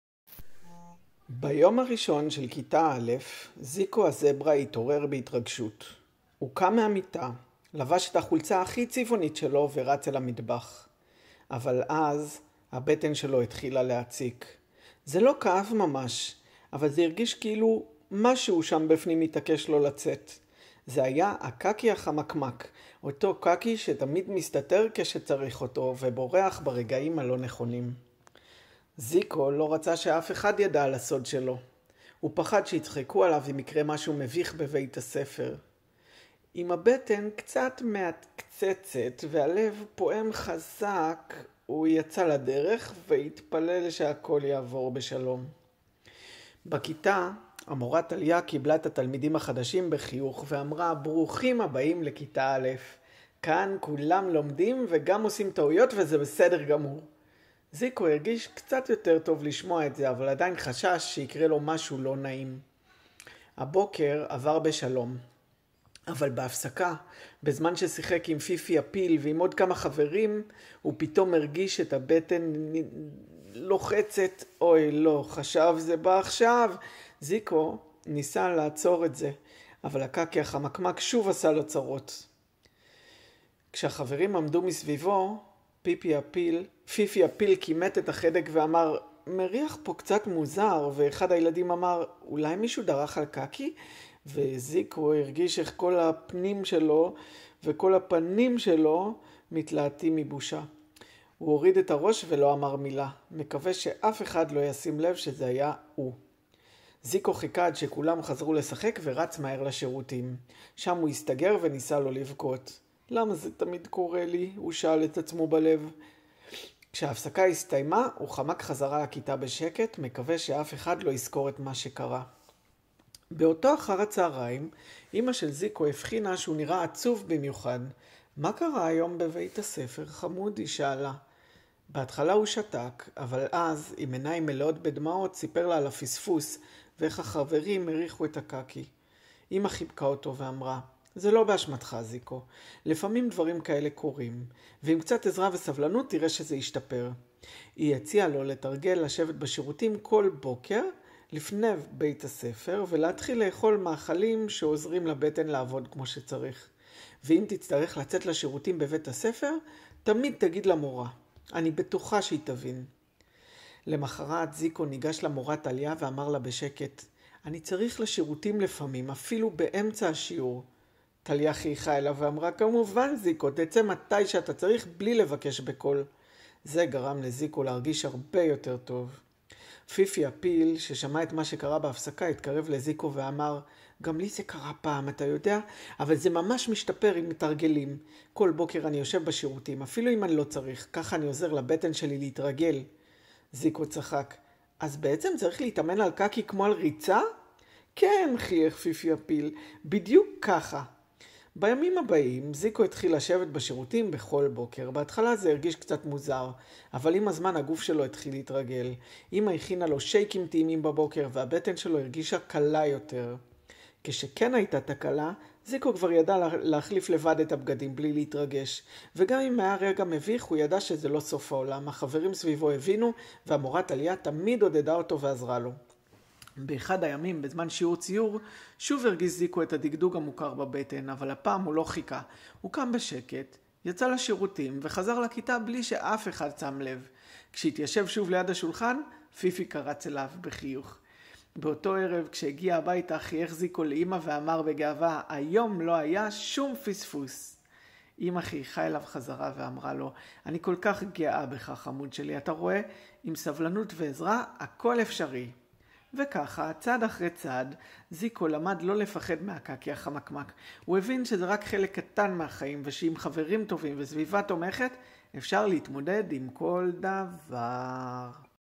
הקקי החמקמק של זיקו הזברה
קשיי גמילה אצל ילדים, אנקופרזיס או הִצְטאוּת, סיפור לילדים